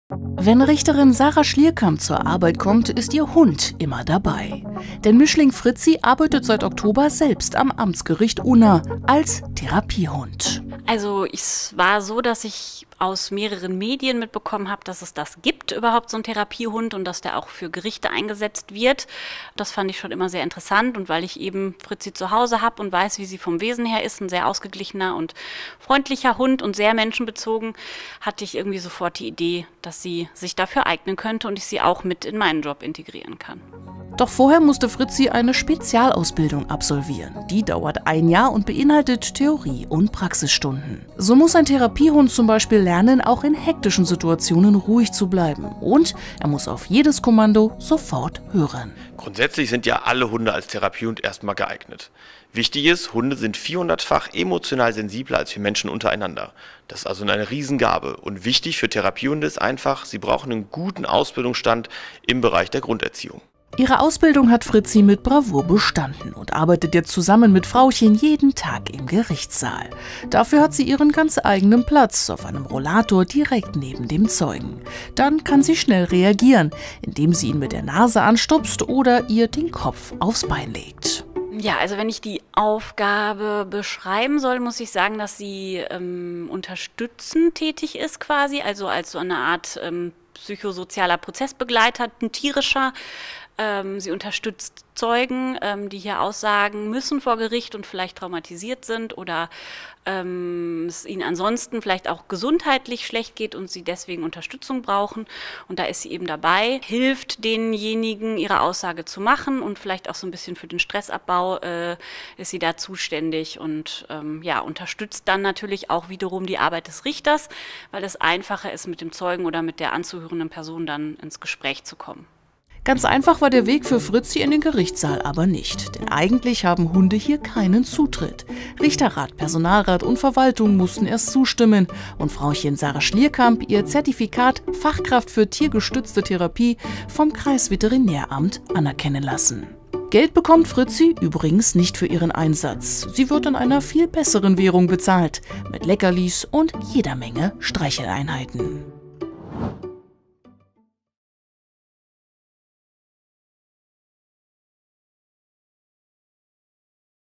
Mit großer Bedeutung: Dieser Mischling hat jetzt schon einen Job
Wenn Sarah Schlierkamp zur Arbeit kommt, ist ihre Fritzi immer dabei – als Therapiehund! Also eine ganz besondere Aufgabe.

Wenn Zeugen vor Gericht verstört sind, Angst haben und Beruhigung brauchen ist Hund Fritzi immer zur Stelle.